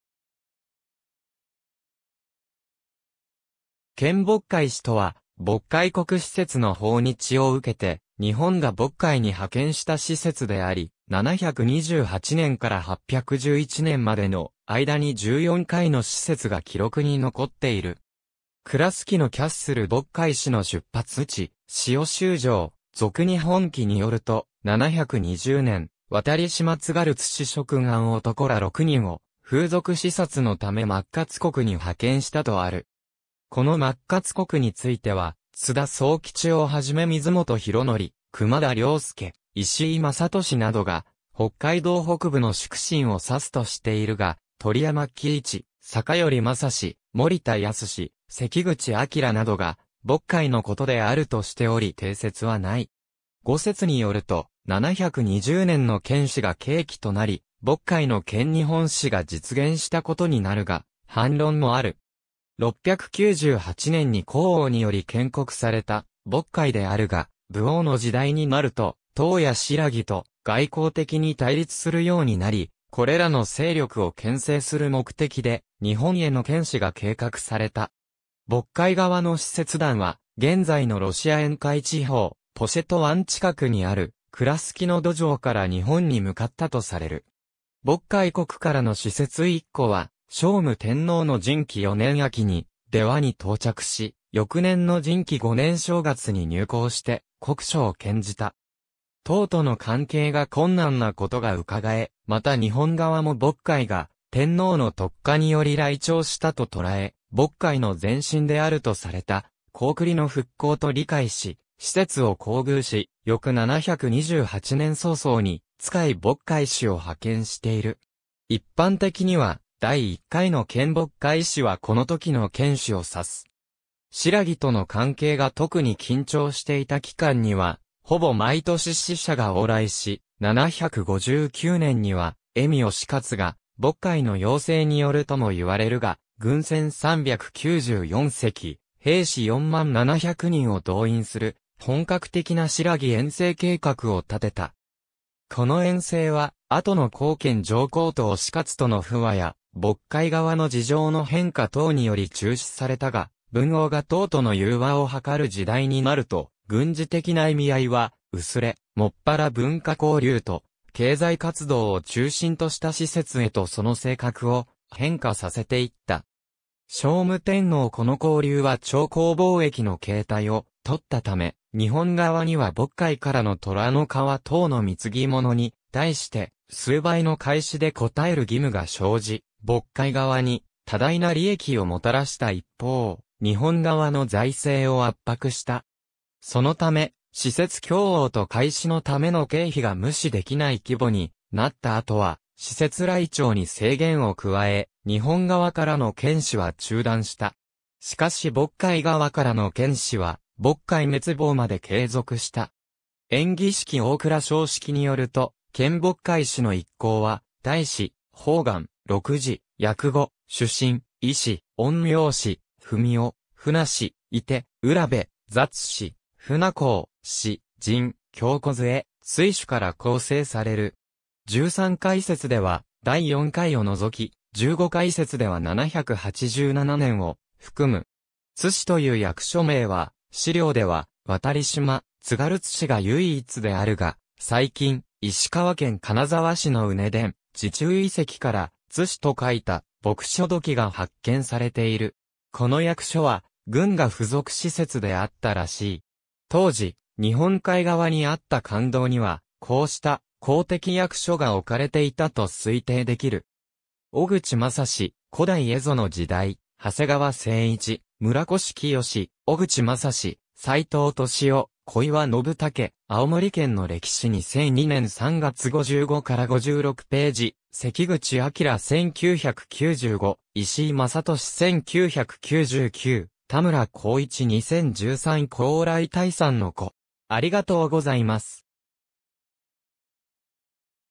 遣渤海使
遣渤海使（けんぼっかいし）とは、渤海国使節の訪日を受けて日本が渤海に派遣した使節であり、728年から811年までの間に14回（または720年から15回。このうち759年は渤海経由の遣唐使。このほか787年、操舵手等の派遣が1回）の使節が記録に残っている。
Kraskino Castle 渤海使の出発地・塩州城（クラスキノ土城） 『続日本紀』によると、720年（養老4年）渡嶋津軽津司諸君鞍男（もろのきみくらお）ら6人を風俗視察のため靺鞨（まっかつ）国に派遣したとある。
この靺鞨国については、津田左右吉をはじめ水本浩典、熊田亮介、石井正敏などが、北海道北部の粛慎（あしはせ）を指すとしているが、鳥山喜一、酒寄雅志、森田悌、関口明などが渤海のことであるとしており定説はない。
後説によると、720年の遣使が契機となり渤海の遣日本使が実現したことになるが、反論もある。
698年に高王により建国された渤海であるが、武王の時代になると唐や新羅と外交的に対立するようになり、これらの勢力を牽制する目的で日本への遣使が計画された。
渤海側の使節団は、現在のロシア沿海地方ポシェト湾近くにあるクラスキノ土城（塩州城）から日本に向かったとされる。
渤海国からの使節一行は、聖武天皇の神亀4年（727年）秋に出羽に到着し、翌年の神亀5年（728年）正月に入貢して国書を献じた。
唐との関係が困難なことが窺え、また日本側も渤海が天皇の徳化により来朝したと捉え、渤海の前身であるとされた高句麗の復興と理解し、使節を厚遇し、翌728年早々に遣渤海使を派遣している。
一般的には第1回の遣渤海使はこのときの遣使を指す。
新羅との関係が特に緊張していた期間（758年-763年）には、ほぼ毎年使者が往来し、759年（天平宝字3年）には恵美押勝が渤海の要請によるとも言われるが軍船394隻、兵士4万700人を動員する本格的な新羅遠征計画を立てた。
この遠征は後の孝謙上皇と押勝との不和や渤海側の事情の変化等により中止されたが、文王が唐との融和を図る時代になると軍事的な意味合いは薄れ、専ら文化交流と経済活動を中心とした使節へとその性格を変化させていった。
聖武天皇 この交流は朝貢貿易の形態を取ったため、日本側には渤海からの虎の皮等の貢物に対して数倍の回賜で応える義務が生じ、渤海側に多大な利益をもたらした一方、日本側の財政を圧迫した。
そのため、使節供応と回賜のための経費が無視できない規模になった後は、使節来朝に制限を加え、日本側からの遣使は中断した。
しかし渤海側からの遣使は渤海滅亡まで継続した。
『延喜式』大蔵省式によると、遣渤海使の一行は、大使、判官、録事、訳語、主神、医師、陰陽師、史生、船師、射手、卜部、雑使、船工、柂師、傔人、挟杪、水手から構成される。
13回説では第4回を除き、15回説では787年（又は720年）を含む。
^ 津司（つし）という役所名は、史料では渡嶋・津軽津司が唯一であるが、最近、石川県金沢市の畝田（うねだ）・寺中（じちゅう）遺跡から「津司」と書いた墨書土器が発見されている。
この役所は郡衙付属施設であったらしい。
当時、日本海側にあった官道にはこうした公的役所が置かれていたと推定できる。
小口雅史「古代蝦夷の時代」　長谷川成一・村越潔・小口雅史・斉藤利男・小岩信竹『青森県の歴史』2002年3月　55-56ページ ^ 関口明　1995 ^ 石井正敏　1999 ^ 田村晃一 2013 ^ 高麗大山の子